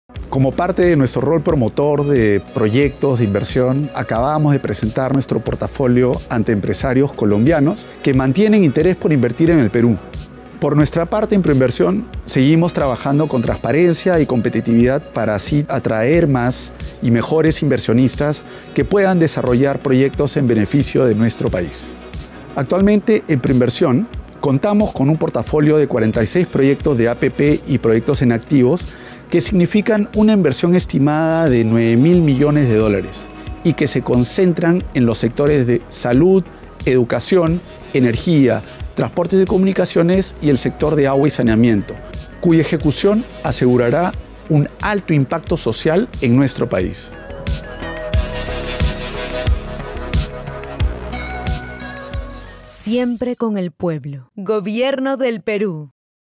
Invest Day - Perú Colombia 2022
PROINVERSIÓN se presento en el Invest Day Perú Colombia 2022 organizado por PromPerú. Presentó su cartera de 46 proyectos APP y PA por una inversión aproximada de US 9,000 millones.